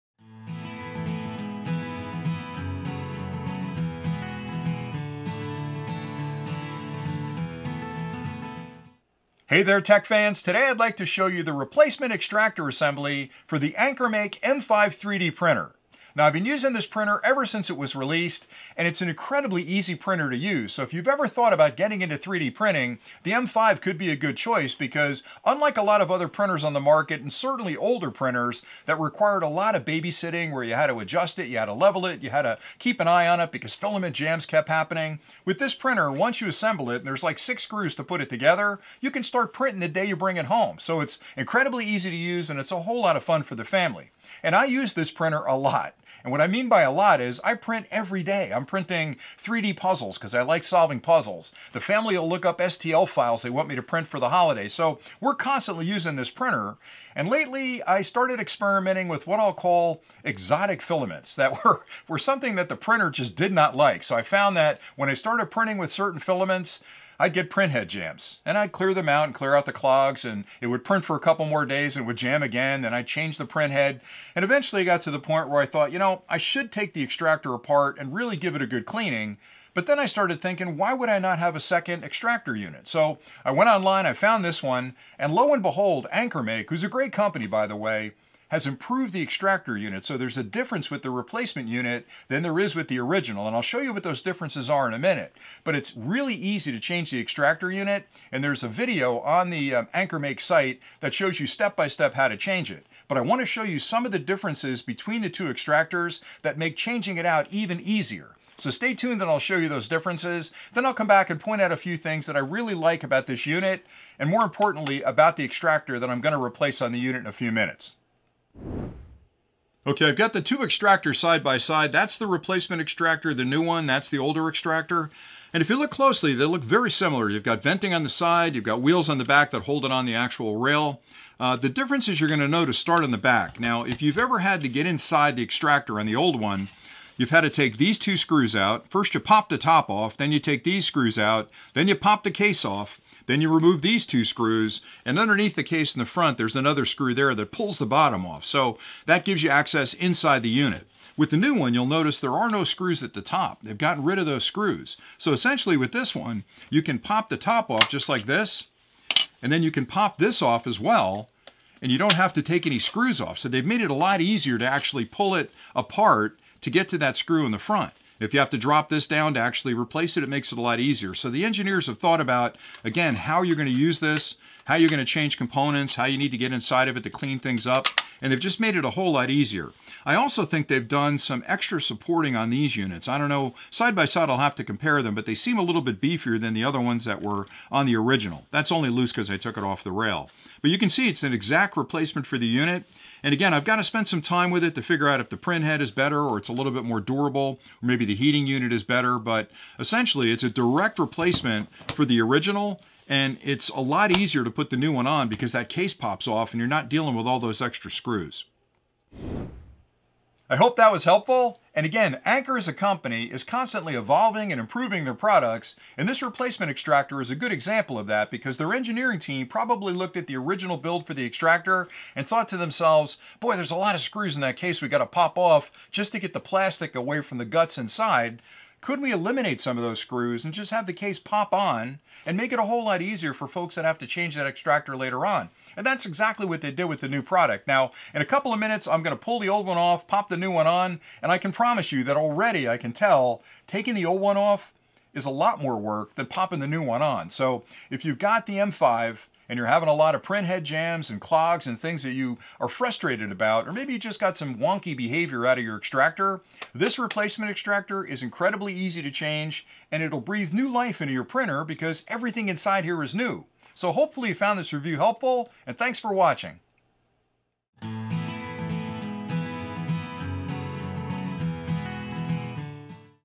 Ankermake M5C Extruder - Great To Have a Spare!
AnkerMake M5 Printer
AnkerMake M5 Extruder

I had a clogged extruder on my Ankermake M5 printer and in this video I show you the replacement unit and explain why I think having a spare is a great idea.